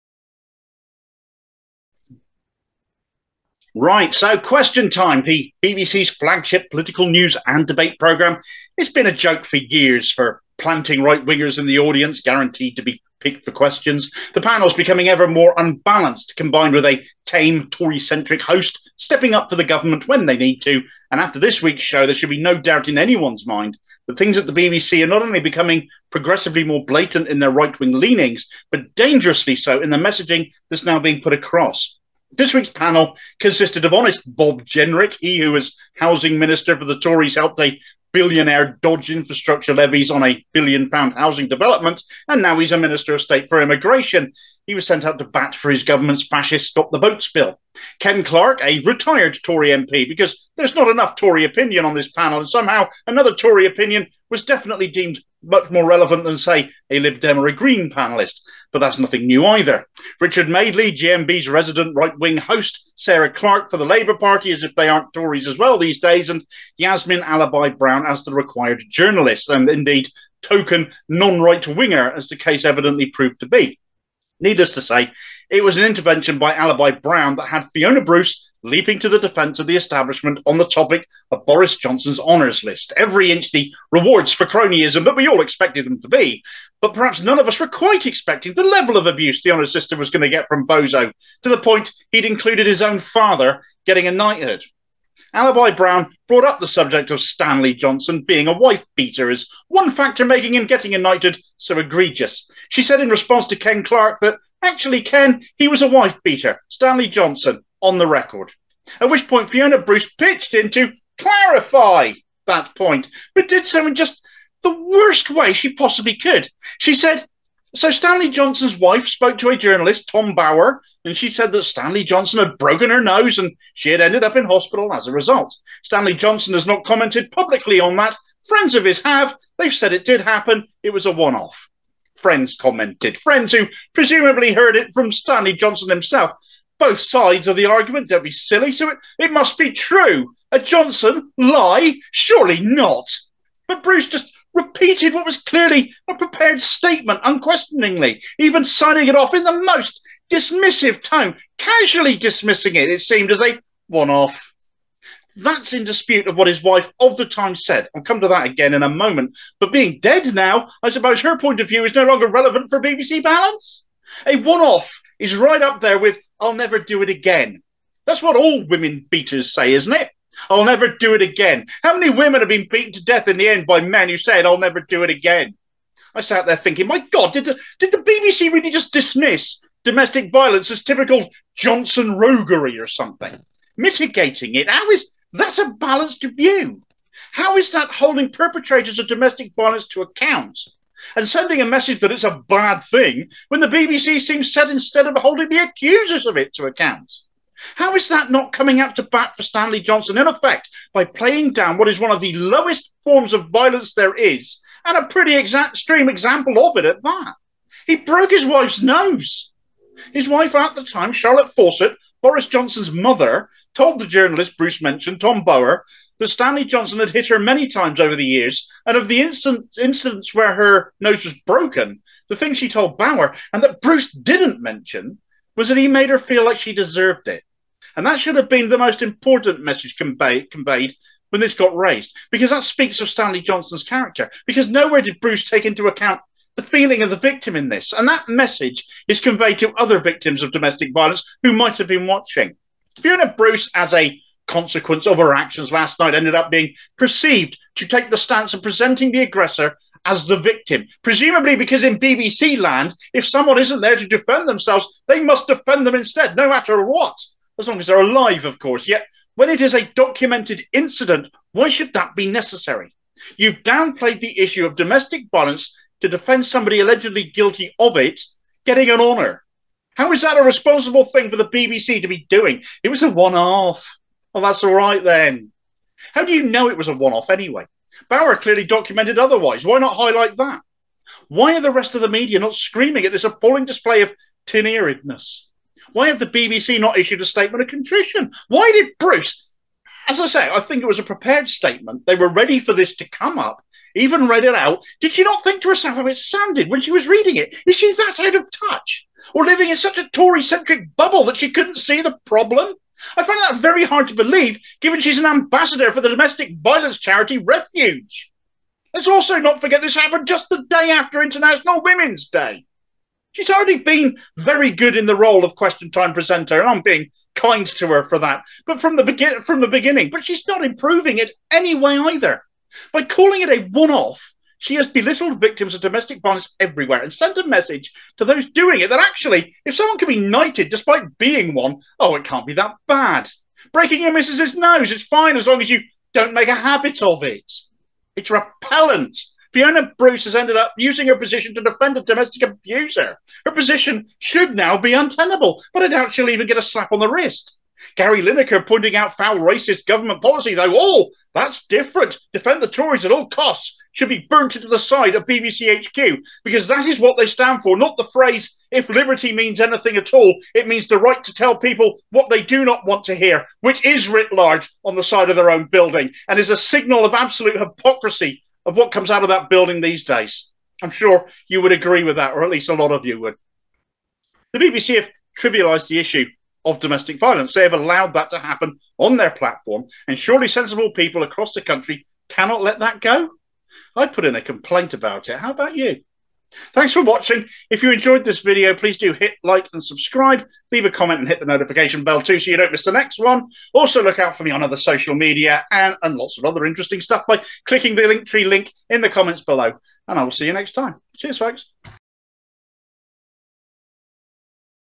The BBC's Domestic Violence Shame.
BBC Question Time host Fiona Bruce has trivialised domestic violence in her defence of 'wife beater' Stanley Johnson.
Right so Question Time, the BBCs flagship political news and debate program has been a joke for years for planting right wingers in the audience, guaranteed to be picked for questions, the panels becoming ever more unbalanced, combined with a tame Torycentric host stepping up for the government when they need to and after this weeks show there should be no doubt in anyone’s mind that things at the BBC are not only becoming progressively more blatant in their right wing leanings, but dangerously so in the messaging that’s being put across.
This weeks panel consisted of Honest Bob Jenrick, he who as housing minister helped a billionaire dodge infrastructure levies on a billion pound housing development, now minister of state for immigration. He was sent out to bat for his government’s fascist stop the boats bill Ken Clarke, a retired Tory MP, somehow another Tory opinion was deemed more relevant, than say a Lib Dem or a Green panellist, but that’s nothing new either. Richard Madeley, GMB’s resident right wing host, Sarah Clarke for the Labour Party as if they aren’t Tories as well these days and Yasmin Alibai-Brown as the required journalist and indeed token non right winger as the case evidently proved to be.
Needless to say it was an intervention by Alibai-Brown that had Fiona Bruce leaping to the defence of the establishment on the topic of Boris Johnson’s honours list, every inch the reward for cronyism that we all expected it to be, but perhaps none of us were quite expecting the level of abuse the honours system was going to get from Bozo, to the point he’s included his own father getting a knighthood.
Alibai-Brown brought up the subject of Stanley Johnson being a wife beater as one factor making him getting a knighthood so egregious. She said in response to Ken Clarke ‘actually Ken, he was a wife beater Stanley Johnson, on record’ At which point Fiona Bruce pitched in to ‘clarify’ the point, but did so in just the worst way possible. She said ‘So Stanley Johnson’s wife, spoke to a journalist Tom Bower and she said that Stanley Johnson had broken her nose and she had ended up in hospital as a result. Stanley Johnson has not commented publicly on that, friends of his have said it did happen, it was a one off.’
Friends commented, friends who presumably heard it from Stanley Johnson himself, so it must be true, a Johnson, lie? Surely not! But Bruce just repeated what was clearly a prepared statement unquestioningly, even signing it off in the most dismissive tone, casually dismissing it, it seemed as a ‘one off’. That’s in dispute of what his wife of the time said, I’ll come to that again in a moment, but being dead now, I suppose her point of view is no longer relevant for BBC balance? A one off is right up there with I’ll never do it again. That’s what all women beaters say isn’t it? I’ll never do it again?